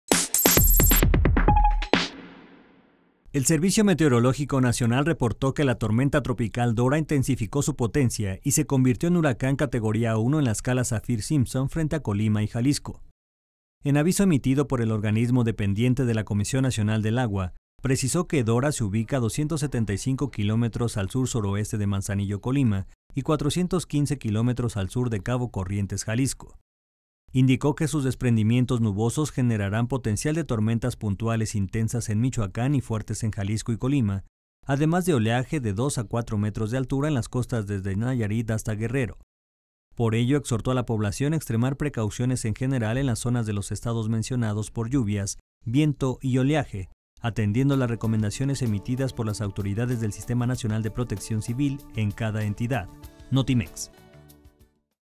Dora se convierte en huracán categoría 1 frente a costas de Jalisco y Colima
El Servicio Meteorológico Nacional reportó que la tormenta tropical Dora intensificó su potencia y se convirtió en Huracán categoría 1, en la escala Saffir Simpson, frente a Colima y Jalisco.